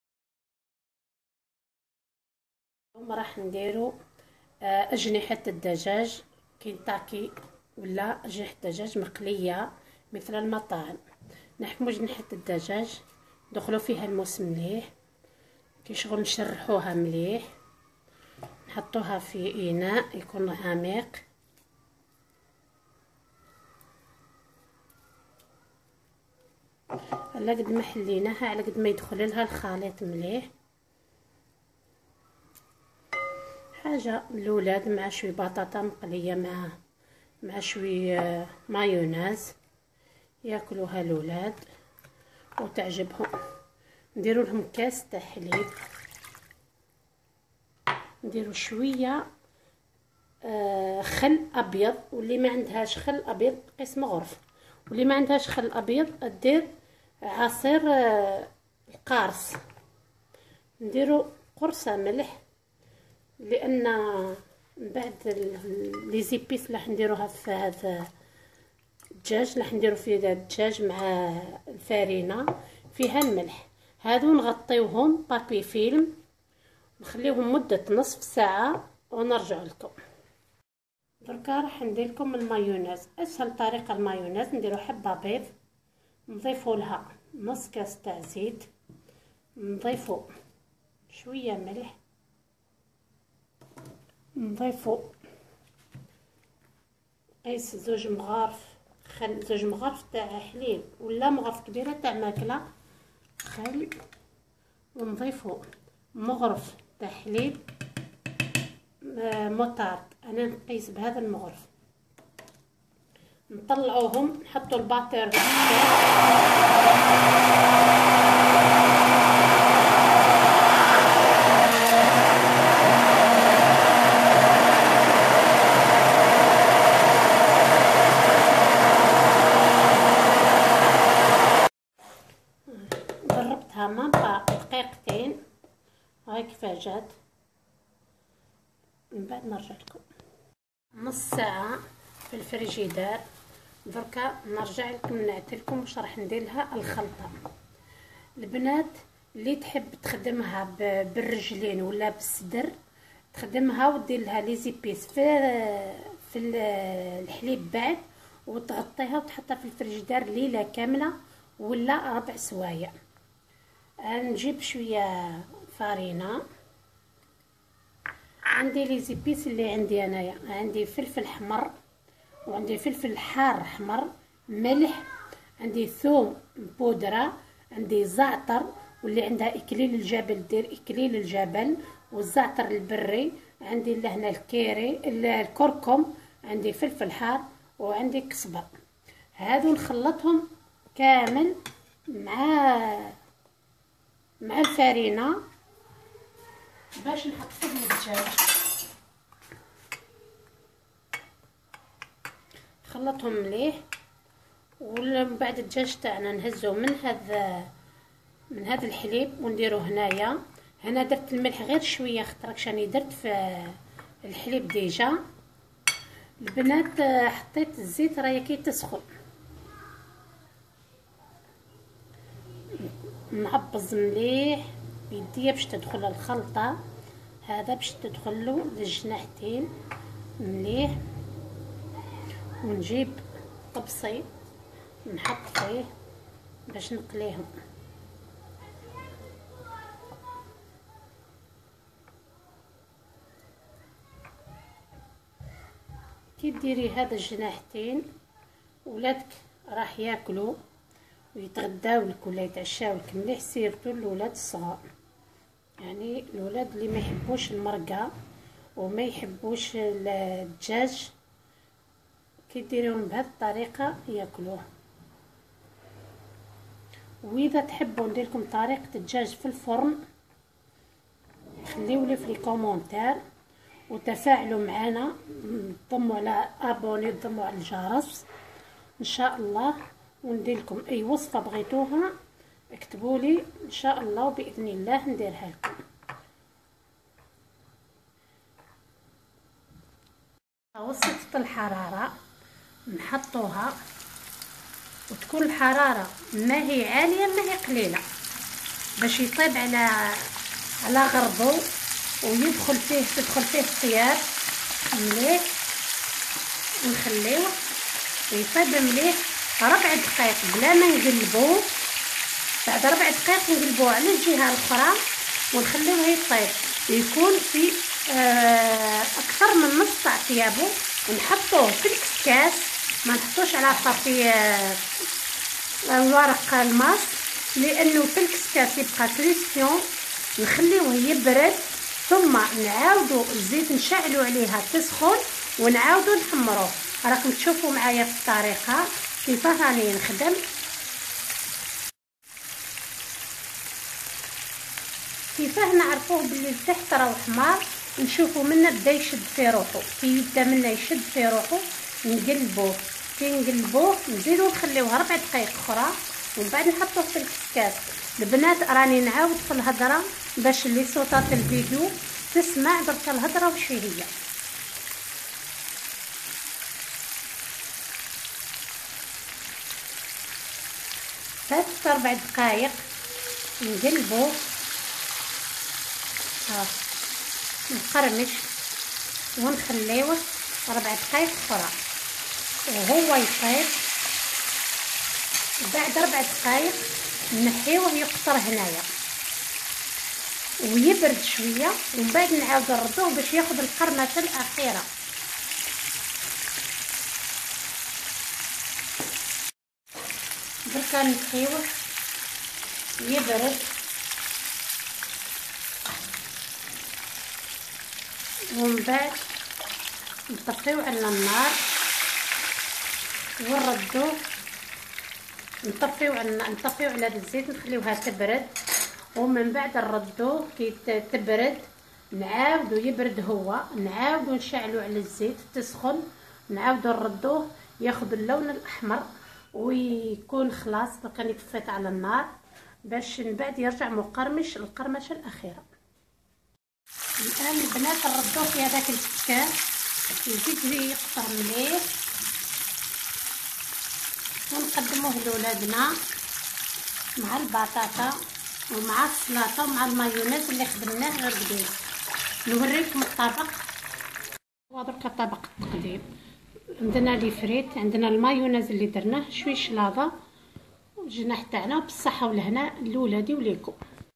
اجنحة دجاج مقلية على الطريقة كينتاكي وتحضير سريع للمايونيز
للاستفسار عن اي مقبلات او حلويات . طبخ او تدابير منزلية...
يترك سؤاله في تعليق .